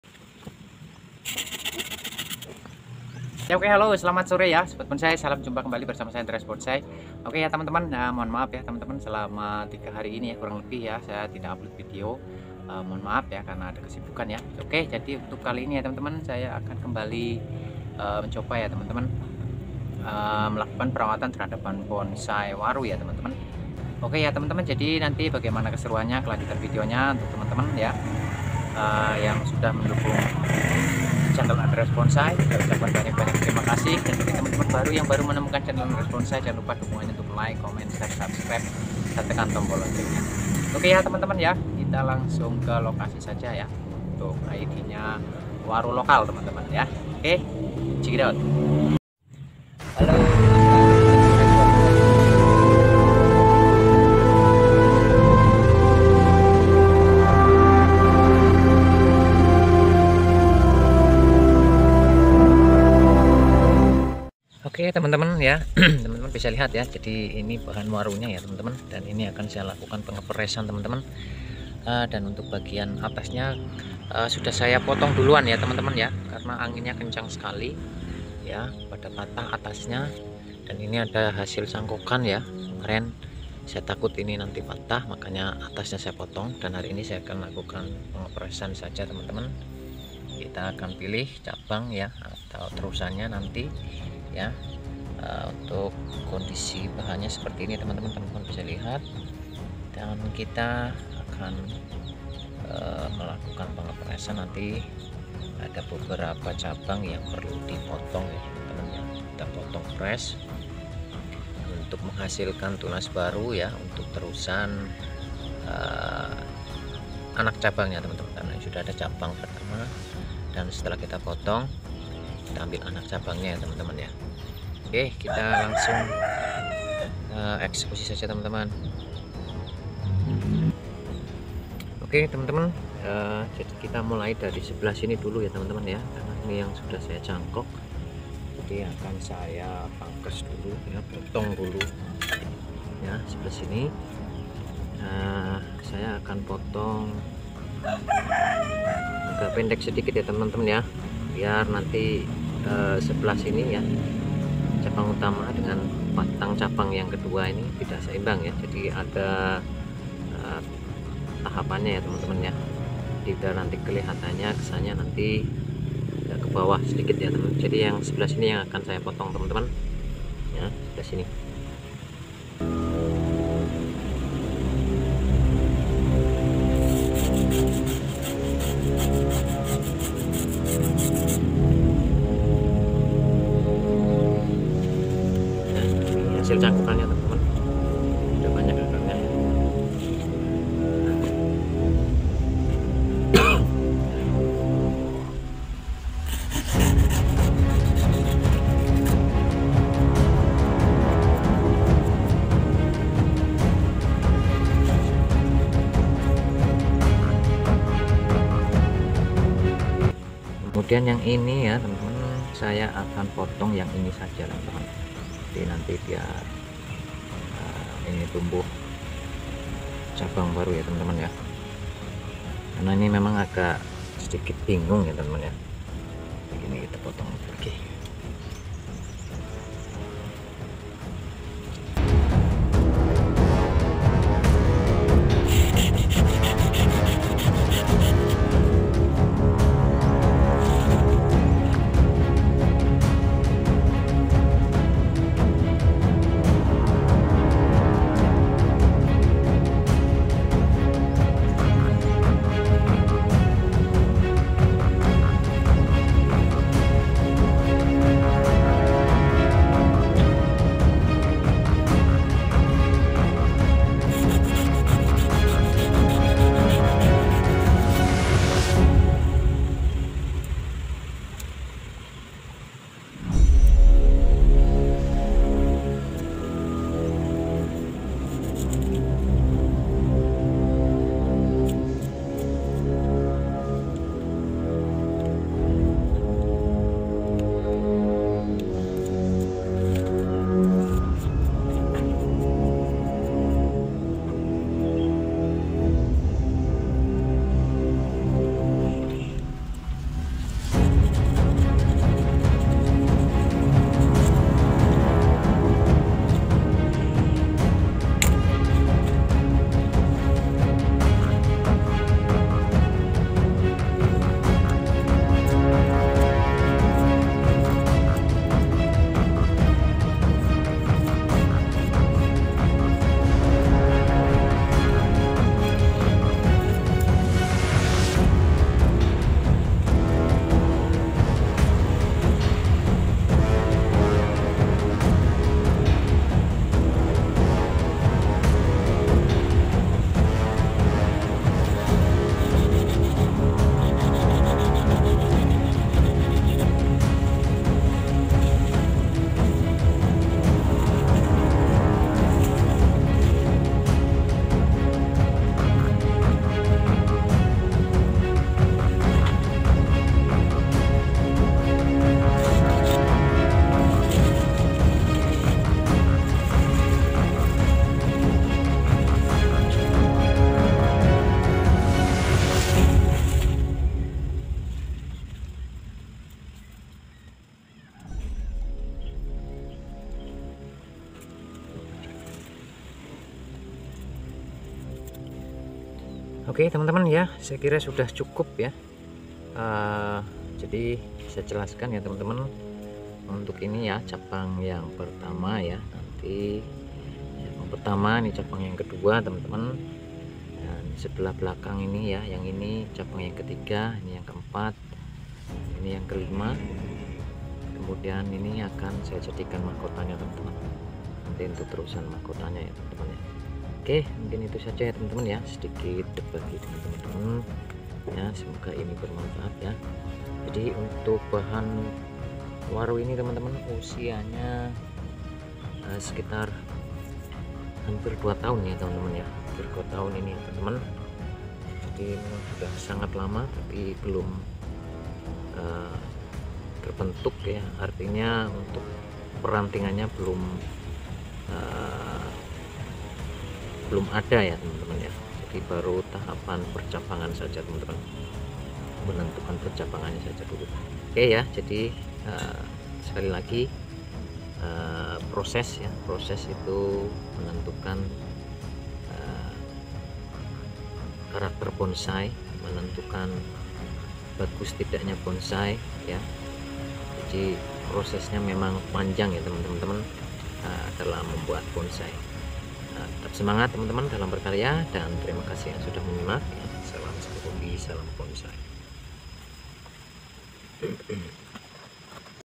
batangnya serem ‼️ tapi blm maksimal.pengepresan cabang waru lokal
selamat menjalankan ibadah puasa bagi teman-teman yang sedang menjalankannya
ok ya kali kesempatan ini saya akan berbagi tips dan cara bagaimana melakukan perawatan terhadap bahan bonsai waru lokal yaitu dengan melakukan pengepresan cabang supaya ada tumbuh tunas untuk terusan anak cabang
yuk kita simak videonya dan bagi teman-teman yang ingin belajar bonsai silahkan teman teman klik link di bawah ini